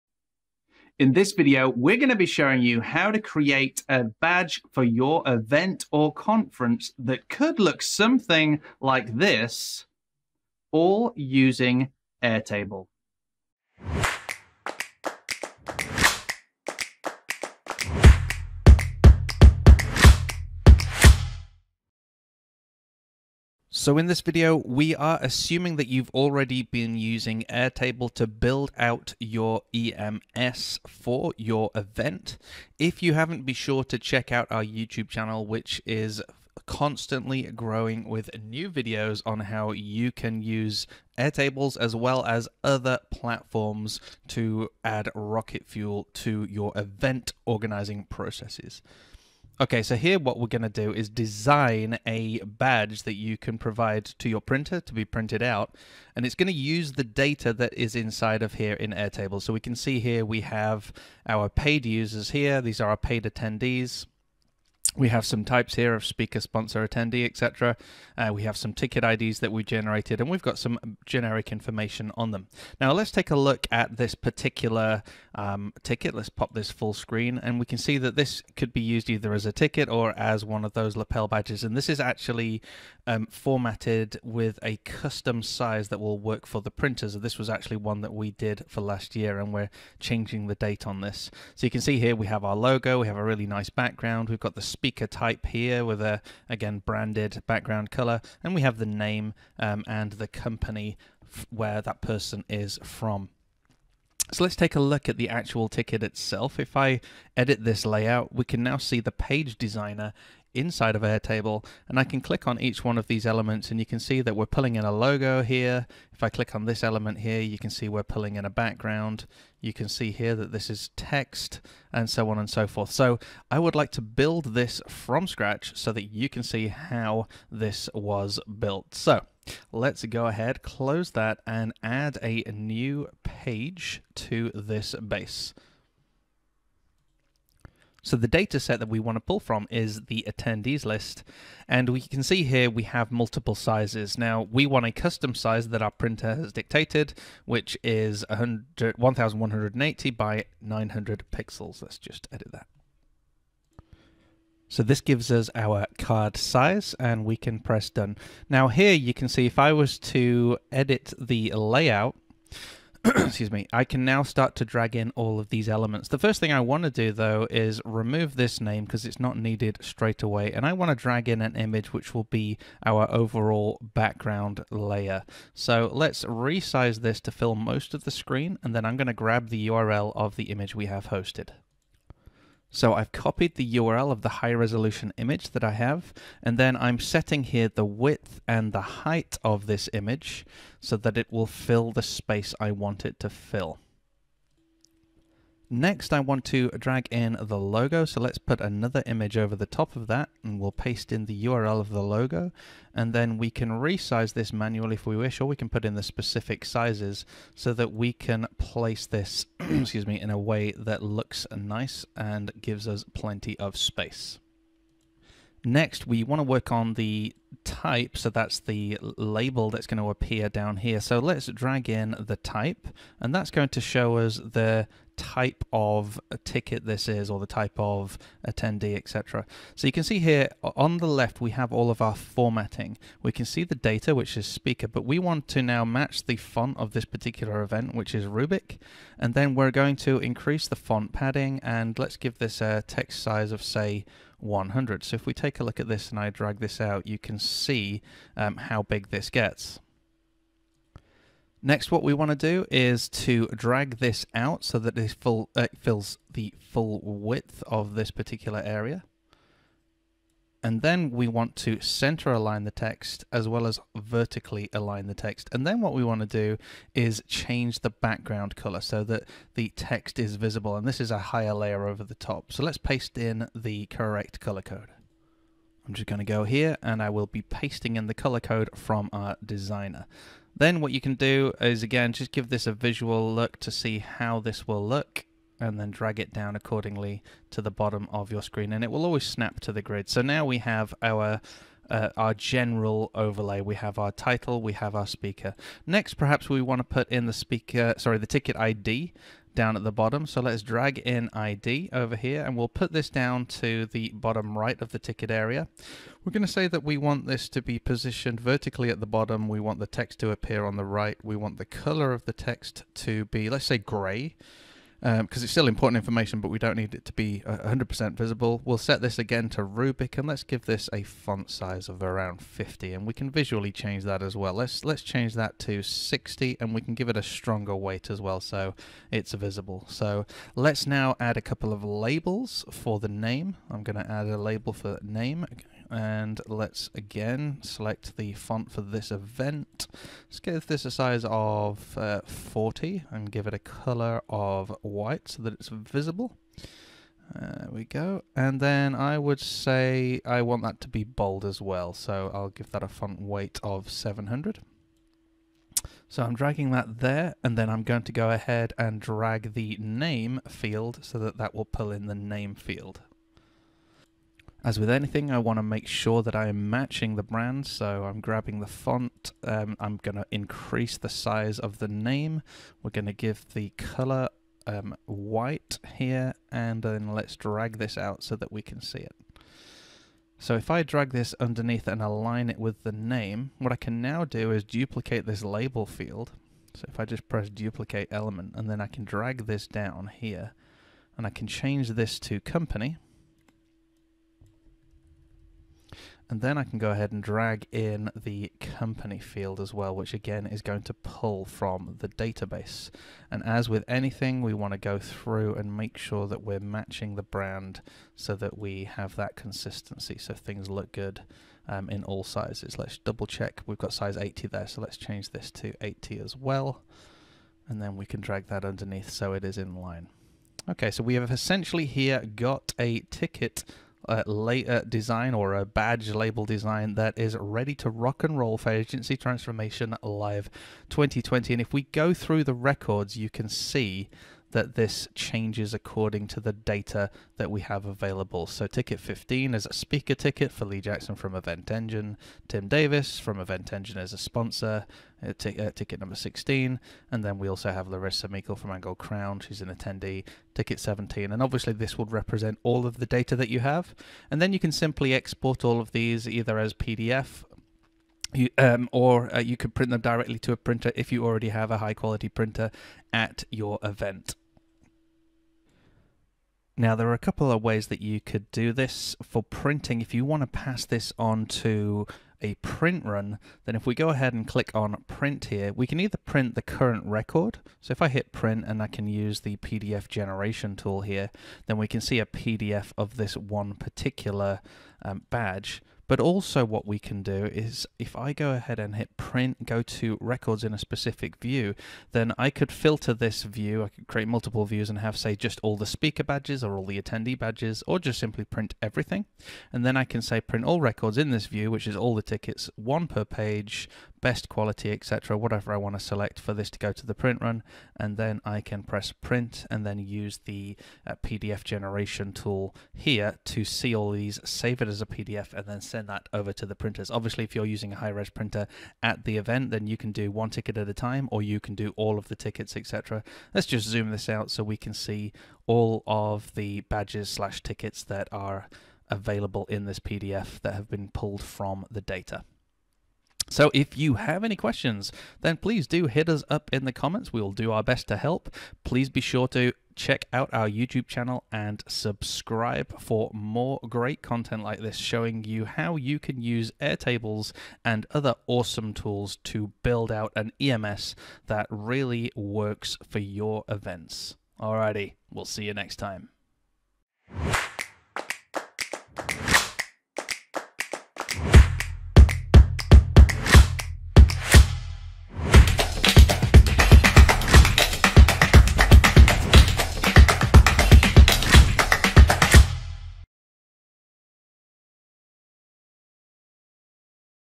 Producing badges in Airtable
Here is a crazy easy way to produce badges and tickets for your conference or event using Airtable.

About Lee:
Co-founder of Event Engine, a WordPress-driven conference website platform designed to help event organisers market to and grow their audience.